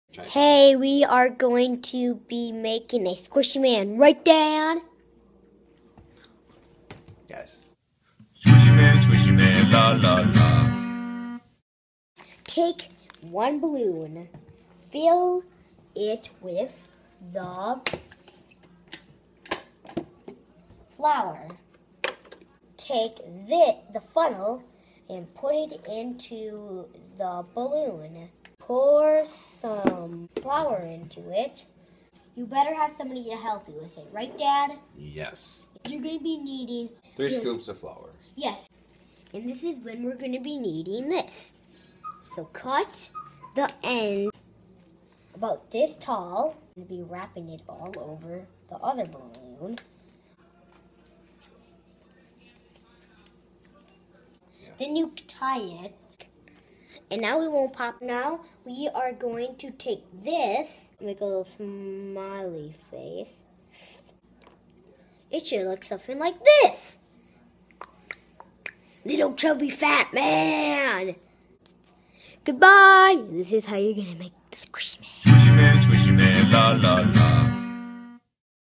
How to Make a Squishy Man
This was one of my first make videos from when I was 6. throwbackthursday (is that a thing still?)